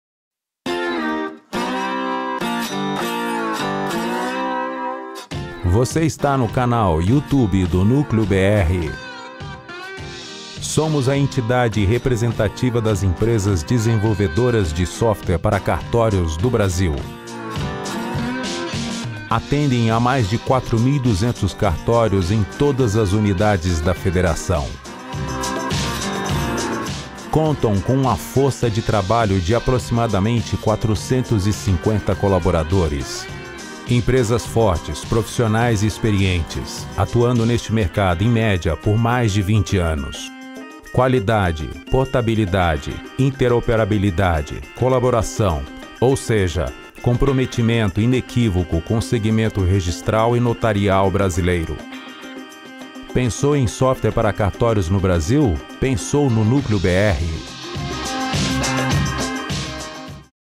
Minha Locução Para um Cliente Abertura Canal Youtube
Fiz no total 7 locuções para este cliente, tudo gravado e produzido em meu estúdio, Power Field Studio com excessão da trilha sonora e do próprio video, neste caso foi a Produtora de Video.